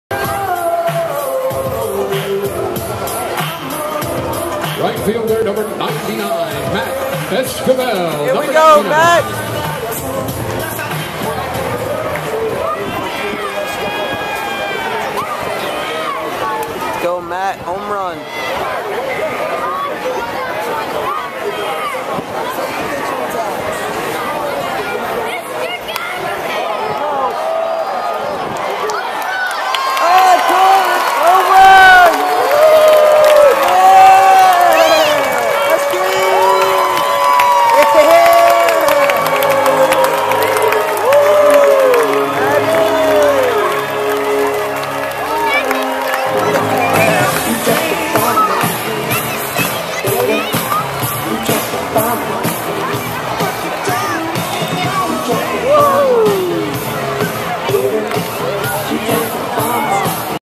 DUCKS MATT ESQUIVEL 3 RUN HOMERUN!!!!!
WOWOWOWOWWOW
MATT ESQUIVEL HITTING A LAZER SHOT
PEDROIA WHO ESQUIVEL=LAZER SHOW